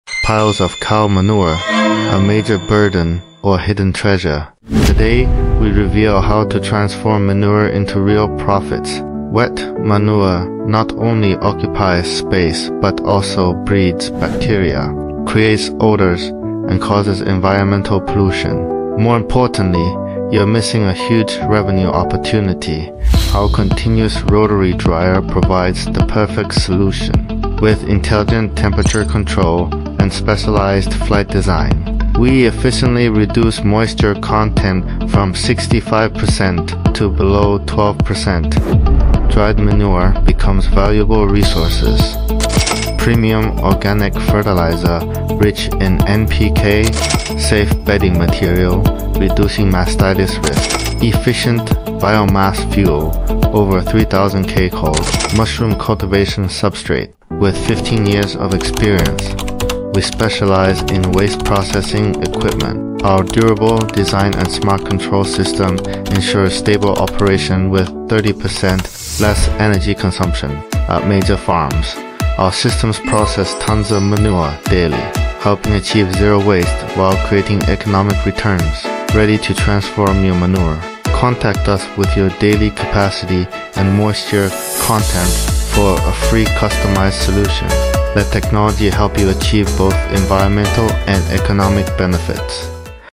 Revolutionizing Manure Management: Complete Drying Solution for Modern Farms
Discover how our continuous rotary dryer transforms cattle manure from an environmental challenge into multiple profit streams. This video demonstrates the complete process from raw material to valuable end products.

The system efficiently converts wet manure (65% moisture) into dry, versatile material ((12% moisture) suitable for:

Commercial organic fertilizer production

Safe and hygienic livestock bedding

High-calorific biomass fuel

Specialty mushroom cultivation

Engineered for agricultural applications, our dryer features intelligent temperature control and specialized flight design, ensuring optimal performance while reducing energy consumption by 30%.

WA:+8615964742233